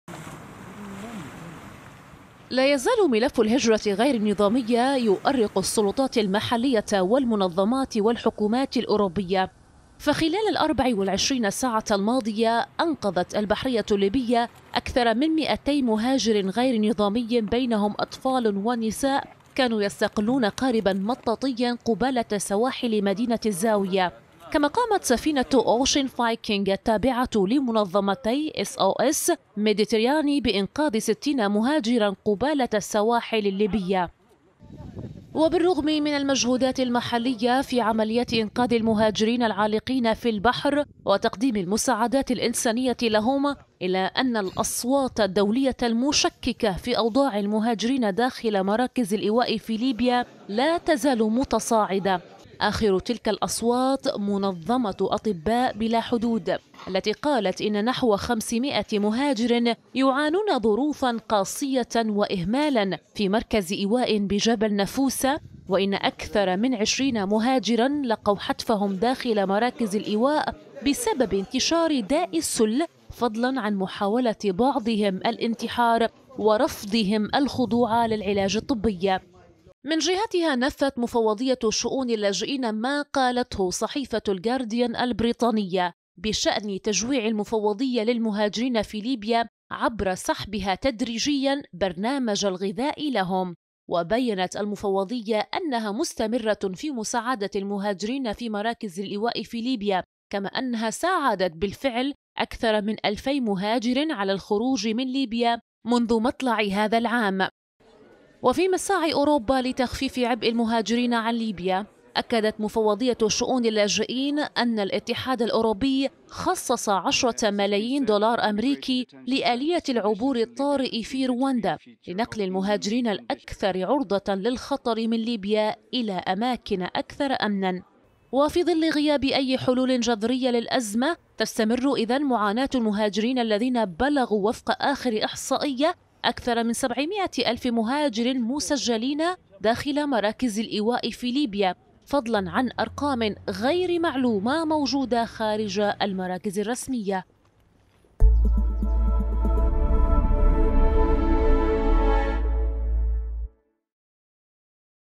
البحرية الليبية تنقذ أكثر من 200 مهاجر قبالة سواحل الزاوية
تقرير  | غالية بوزعكوك